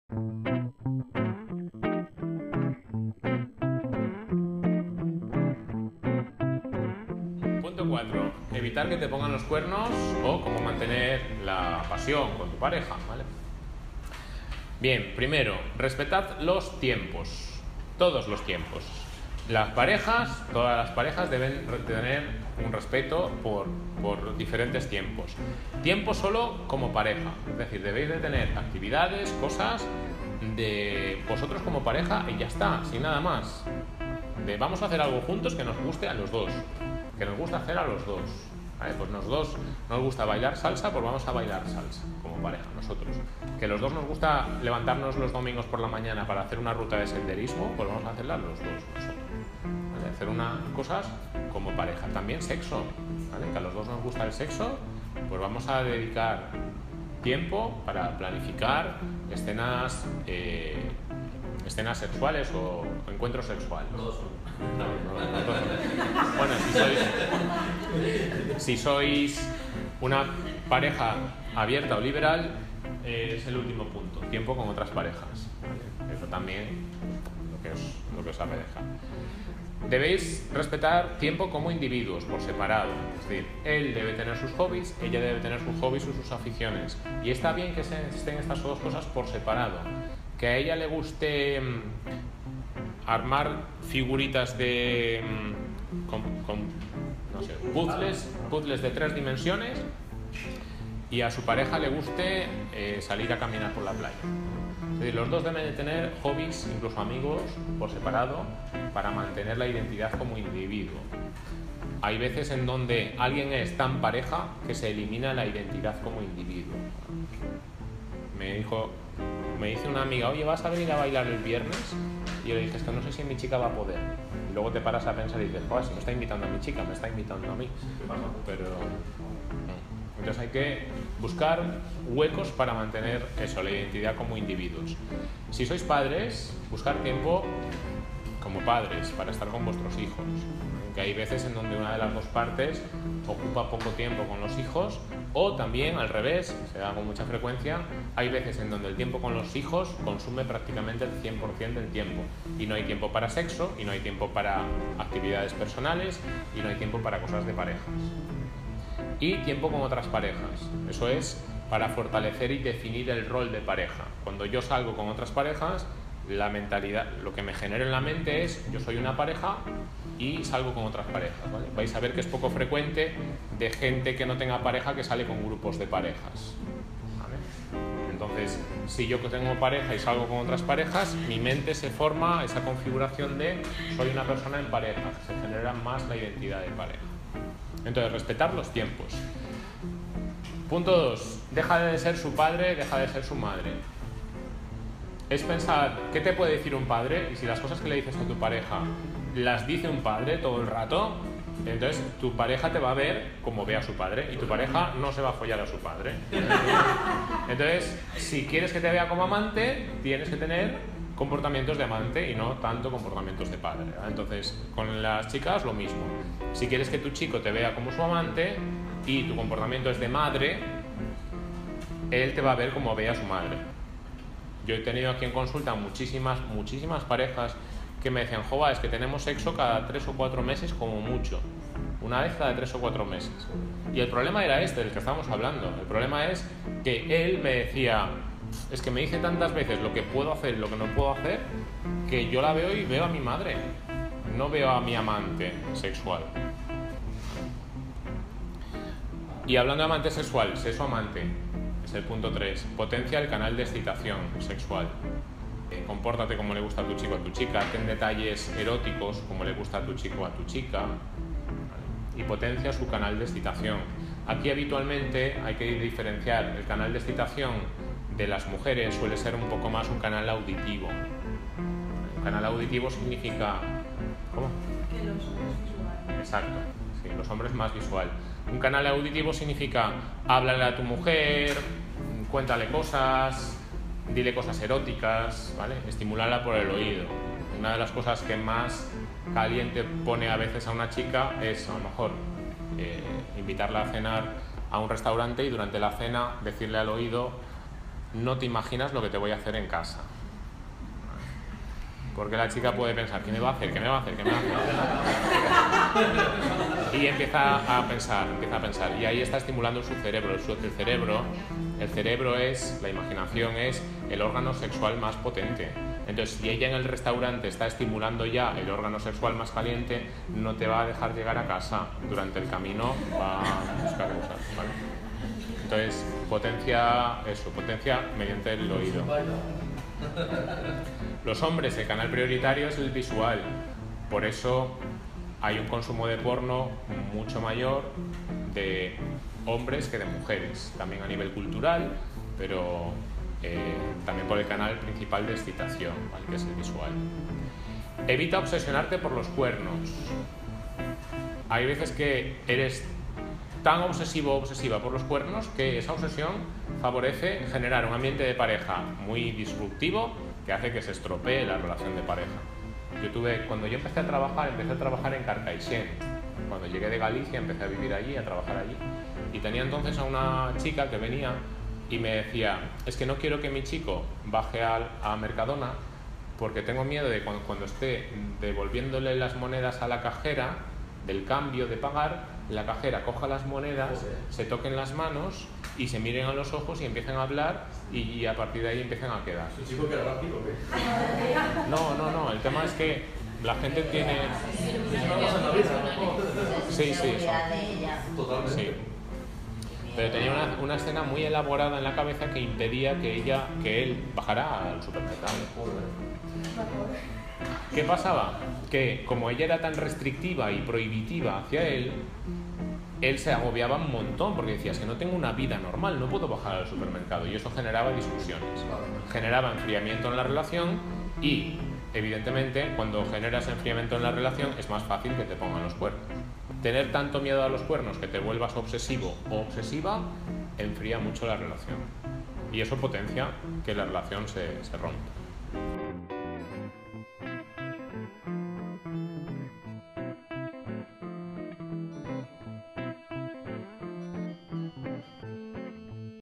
Cómo evitar los cuernos | Psicólogo Fernando Pena
El psicólogo Fernando Pena te da algunas pautas para que tu pareja no te ponga los cuernos.
El psicólogo lleva más de 14 años trabajando con parejas en consulta ayudándoles a tener mejor relación de pareja.
Las infidelidades es uno de los riesgos más altos a la hora de perder una pareja. Hay pautas de comportamiento que fomentan que te sean infiel, y otras que lo dificultan. Conocerlas y aplicarlas te puede ayudar a tener una relación de pareja más estable y satisfactoria.